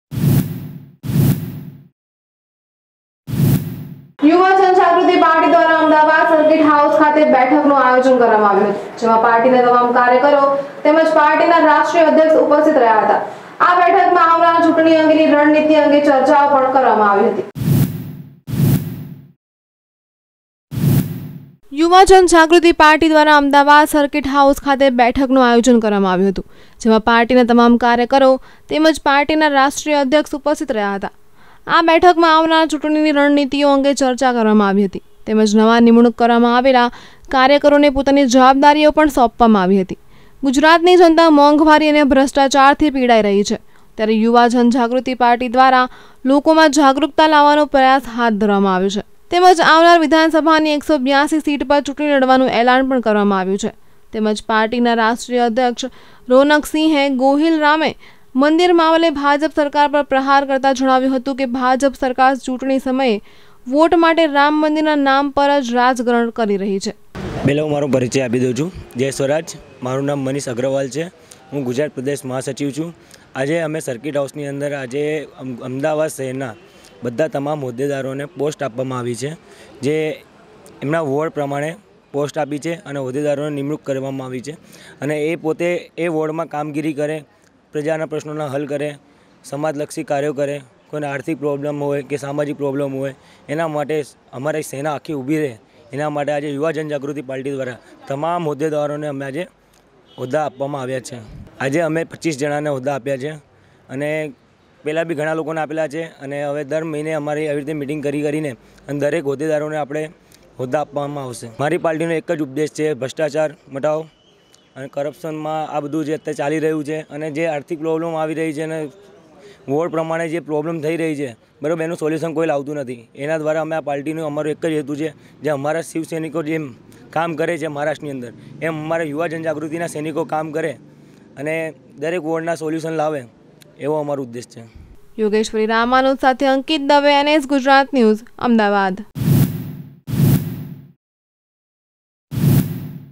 અમદાવાદ યુવા જન જાગૃતિ પાર્ટી દ્વારા અમદાવાદ સર્કીટ હાઉસ ખાતે બેઠકનું આયોજન || NS GUJARAT NEWS
અમદાવાદ યુવા જન જાગૃતિ પાર્ટી દ્વારા અમદાવાદ સર્કીટ હાઉસ ખાતે બેઠકનું આયોજન